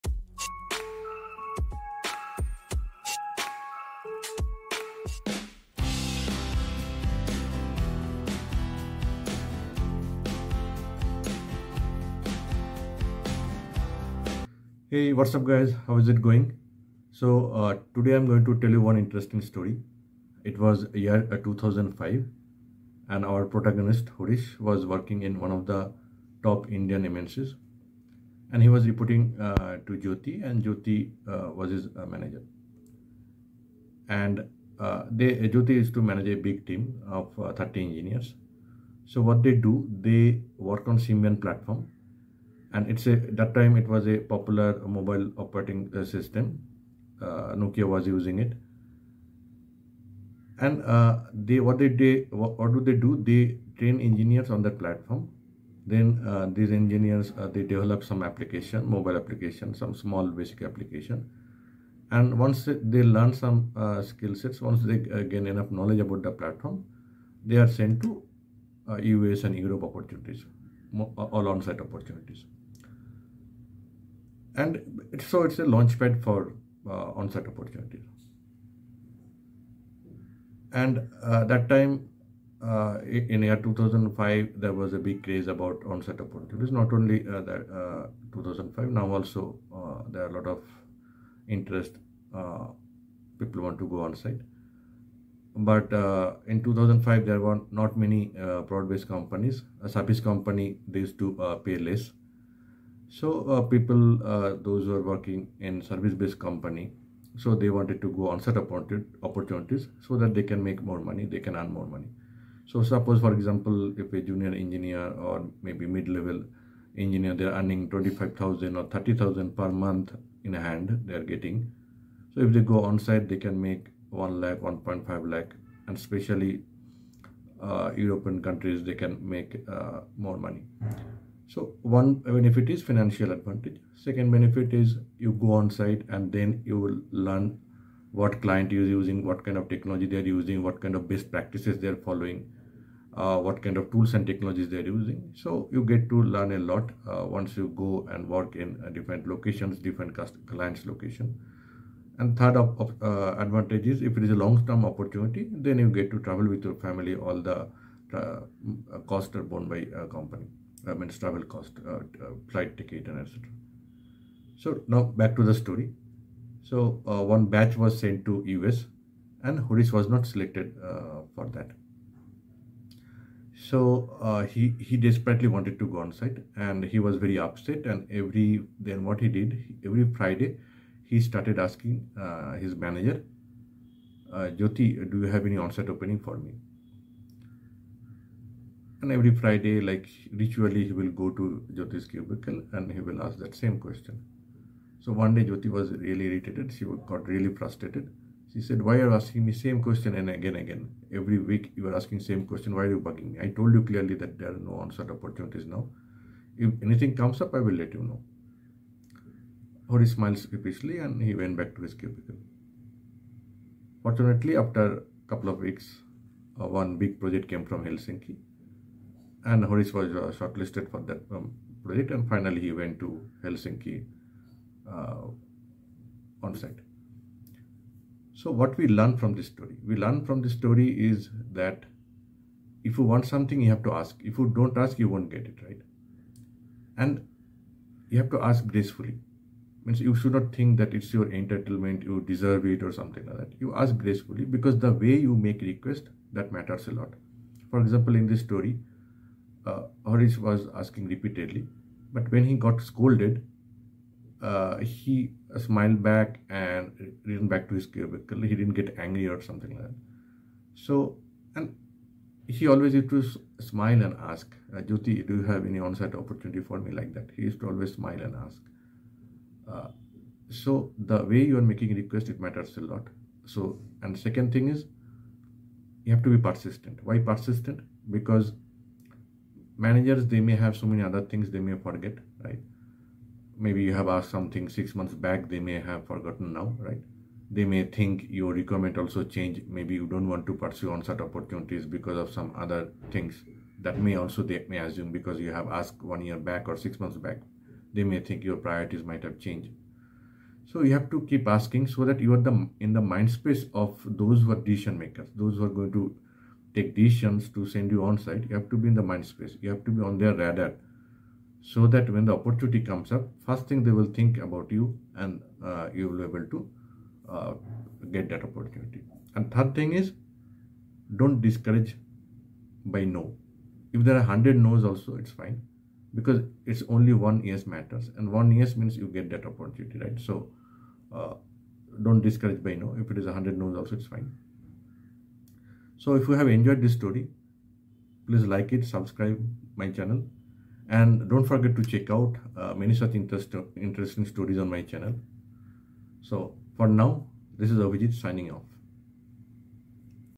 Story 3: Single question which leads to onsite opportunity | Office stories
In the heart of Bengaluru's tech hub,  Harish's relentless quest for an onsite opportunity tests the limits of persistence.
Listen to this story where a SINGLE QUESTION may hold the key to reshaping his destiny forever.

Check out my book for many such stories of cubicle dwellers.
Discover valuable insights to shape your journey.
101 Bedtime Stories for Cubicle Dwellers by Abhijit Gupta